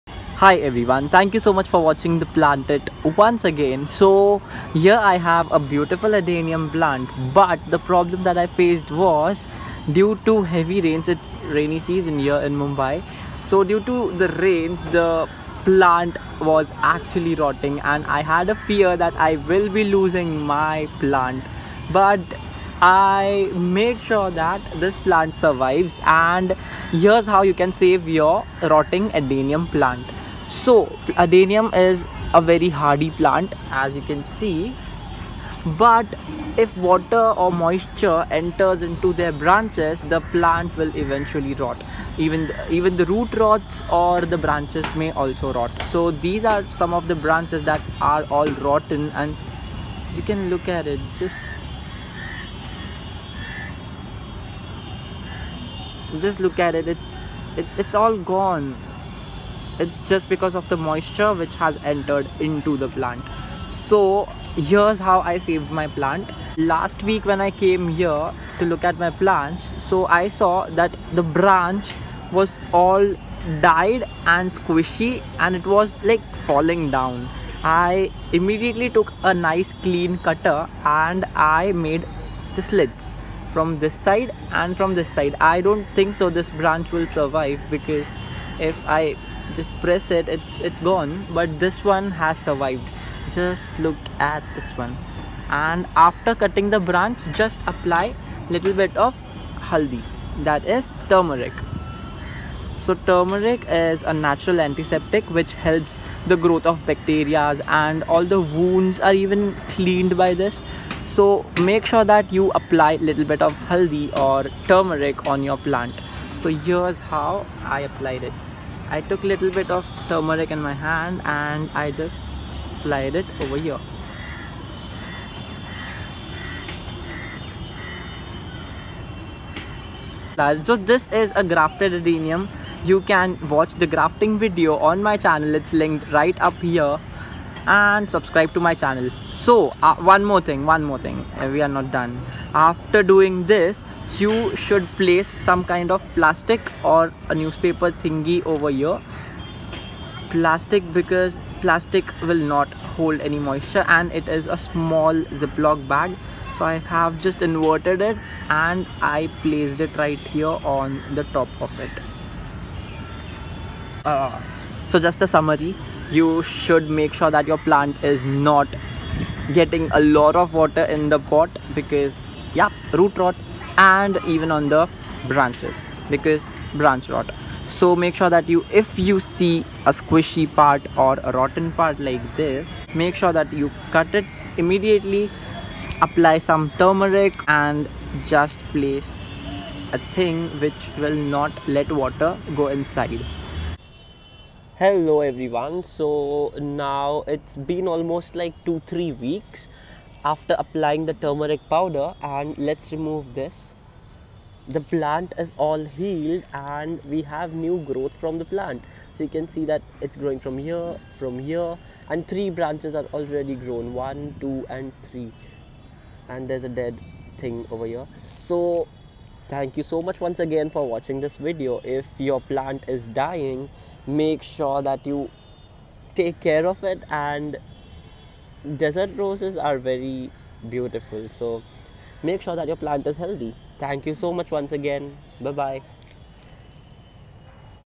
HOW TO SAVE YOUR ADENIUM PLANT FROM ROTTING IN MONSOON
Adenium root or branch rot ? Here’s how you can save it during monsoon